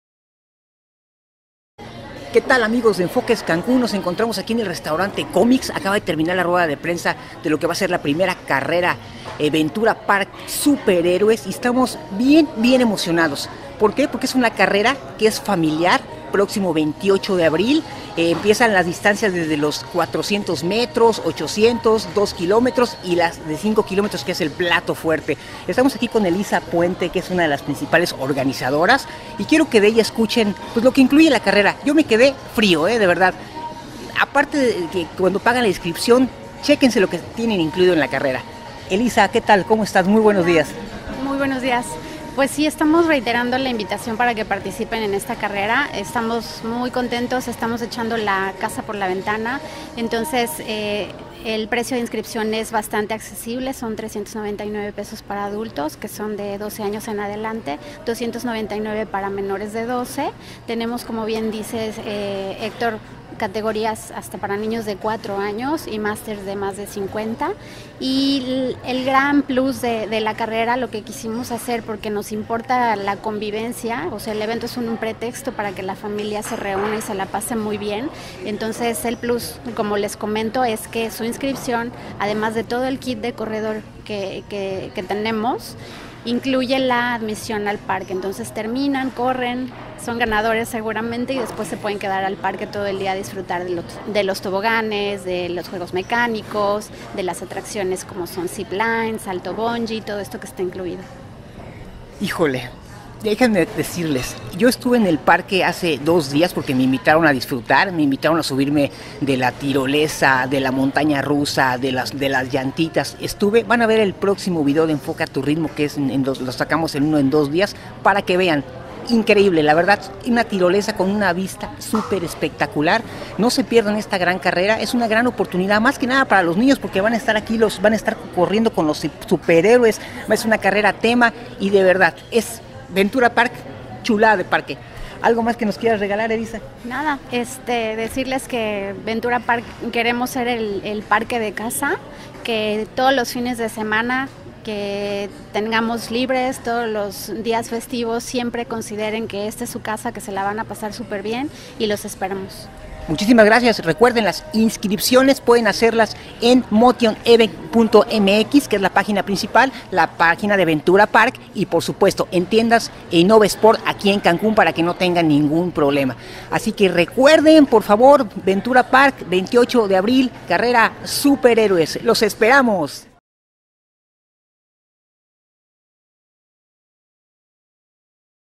Promo Primera Carrera ventura Park
Despues de la rueda de la Primera Carrera ventura park es staff de la carrera nos explica que incluye este magnífico evento.